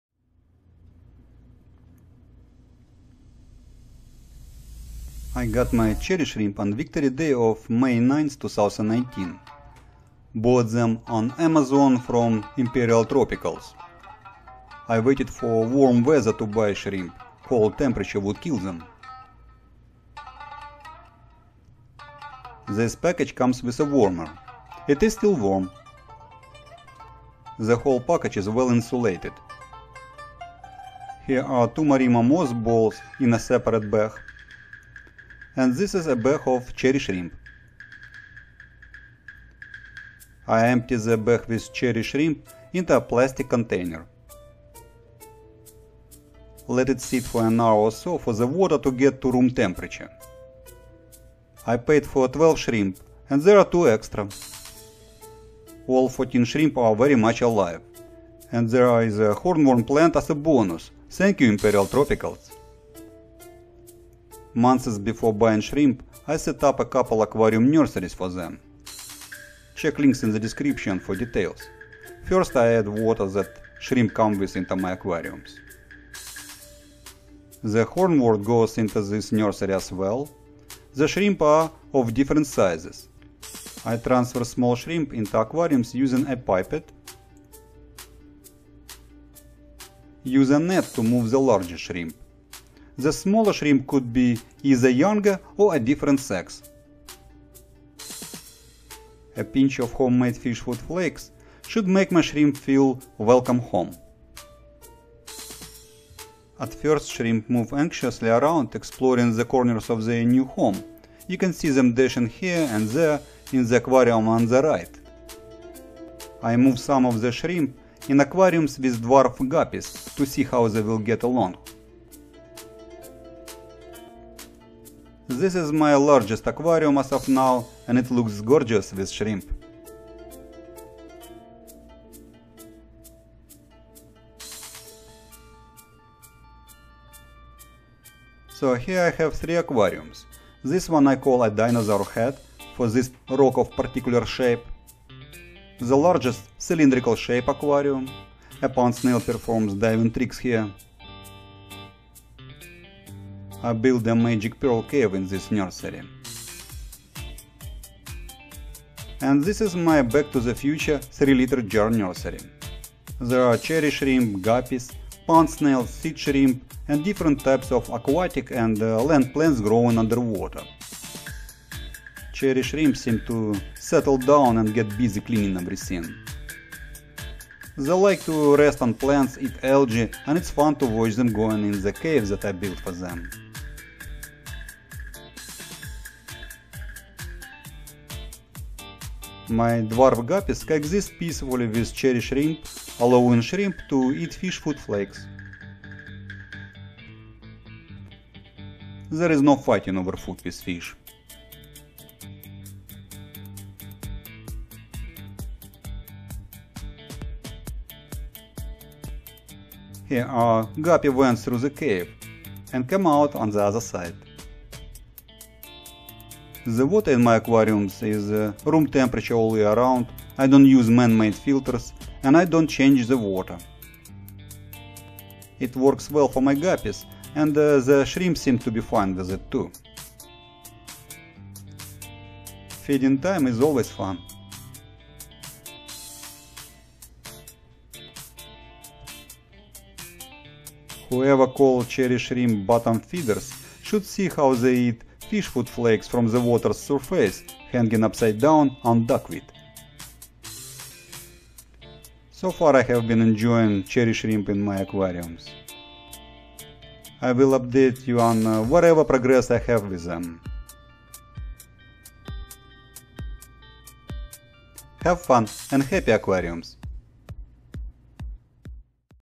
Cherry Shrimp Beginning of Fun
Buy Cherry Shrimp on Amazon (paid link):
00:00 Title
00:05 I got my Cherry shrimp on Victory day of May 9th, 2019
Bought them on Amazon from Imperial
I waited for warm weather to buy shrimp.
Cold temperatures would kill them.
This package comes with a warmer.
It's still warm!
 And this is a bag of Cherry shrimp.
 00:40 I empty the bag with Cherry shrimp into a plastic container.
 Let it sit for an hour or so for the water to get to room temperature.
I paid for 12 shrimp and there are 2 extra.
 All 14 shrimp are very much alive.
 And there is a hornwort plant as a bonus :)
Thanks ImperialTropicals!
01:04 Months before buying shrimp, I set up a couple aquarium nurseries for them
 01:12  First I add water that shrimp came with into my aquariums.
The hornwort goes into this nursery as well.
The shrimp are of different sizes.
01:22 I transfer small shrimp into aquariums using a pipette.
Use a net to move the larger shrimp.
The smaller shrimp could be either younger or a different sex.
01:40 A pinch of homemade fish food flakes should make my shrimp feel welcome home
At first shrimp move anxiously around exploring the corners of their new home.
You can see them dashing here and there in the aquarium on the right.
I move some of the shrimp in aquariums with dwarf guppies to see how they will get along.
02:07 This is my largest aquarium as of now and it looks gorgeous with shrimp :)
So here I have 3 aquariums.
02:22 This one I call a Dinosaur head - for this rock of particular shape
02:35 I build a magic pearl cave in this nursery.
There are Cherry shrimp, guppies, pond snails, seed shrimp, and different type of aquatic and land plants growing underwater
Cherry shrimp seem to settle down and get busy cleaning everything.
03:04 They like to rest on plants, eat algae, and it's fun to watch them going in the caves that I built for them.
My dwarf guppies coexist peacefully with the Cherry shrimp, allowing shrimp to eat fish food flakes.
There is no fighting over food with fish.
03:29 Here a guppy went through the cave... and came out on the other side :)
 The water in my aquariums is room temperature all year around, I don't use man-made filters, and I don't change the water.
 It works well for my guppies and the shrimp seem to be fine with it too.
04:00 Feeding time is always fun.
Whoever called Cherry shrimp bottom feeders should see how they eat fish food flakes from the water’s surface hanging upside down on duckweed.
 So far I have been enjoying Cherry shrimp in my aquariums.
I will update you on whatever progress I have with them.
Have fun and happy aquariums :)